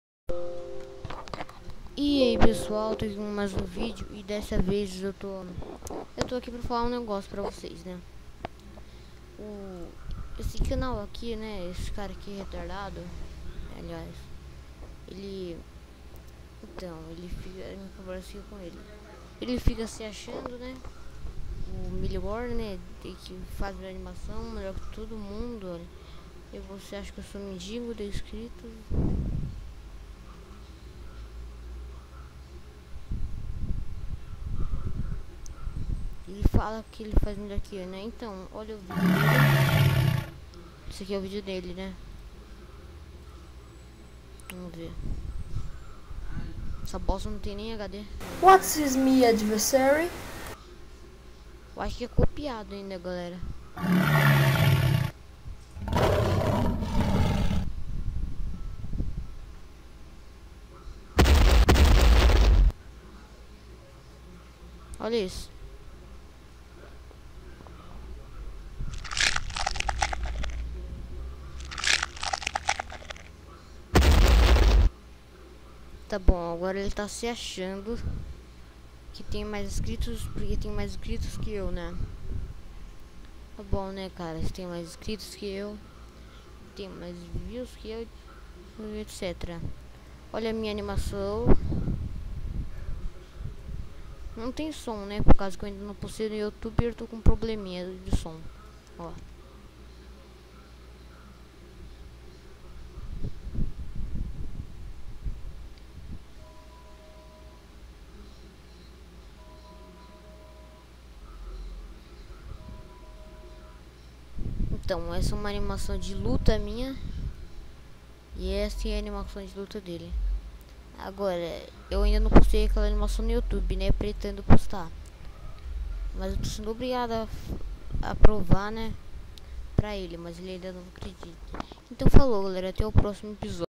Quem Anima Melhor?
Galera eu fiz esse video só pra Ver quem Acha Que anima Melhor Eu ou ou Jake The DogTM Pra quem nao Sabe é um Youtuber Escremento Que Se Acha demais só que ele nao anima Mt Bem eu n Gosto de fazer Videos Assim Mas To Fazendo Porq Esse Cara Ta Enchendo o Saco Só Pq Ele Tem Mais Views e Enscritos Que Eu